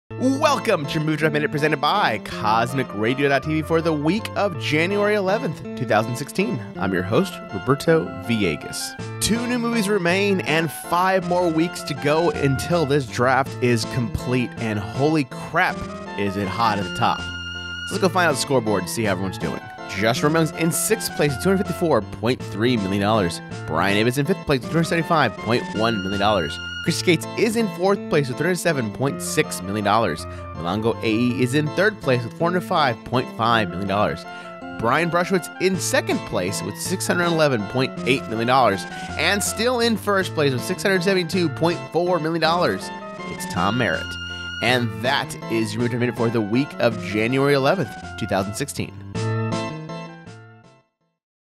Movie Draft Minute - Winter 2015 - Week 15
It's the Season of Star Wars(finally)... and the Movie Draft Minute is here. May the force be with you.

This was created FOR THE PEOPLE OF DIAMOND CLUB! (or Night Attack, but we all know who pays them).